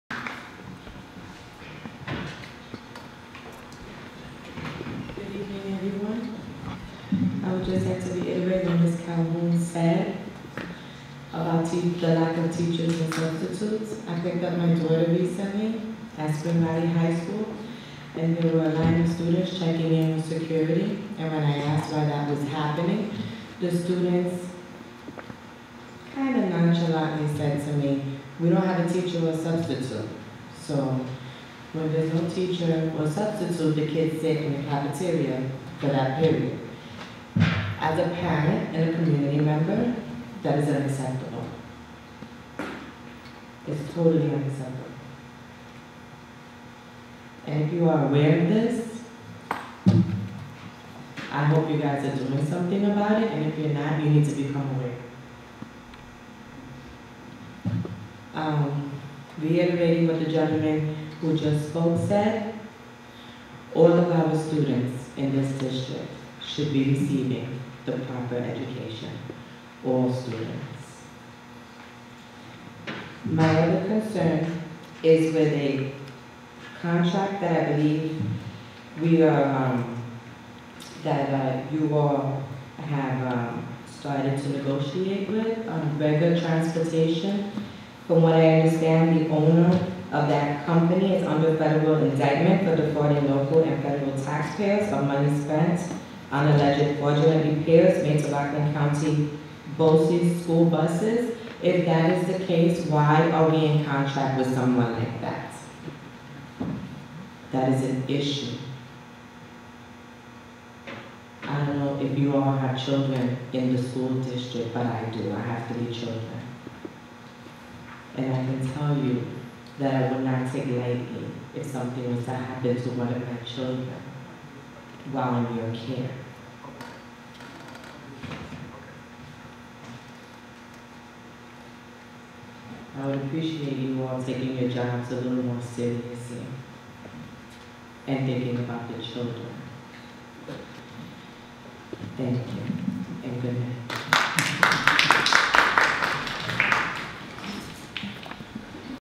East Ramapo Chevon Dos Reis speaks about children
East Ramapo parent Chevon Dos Reis speaks about the lack of substitutes in the high schools. What is this contract wit Brega Transportation?

video from the April 19,2017 meeting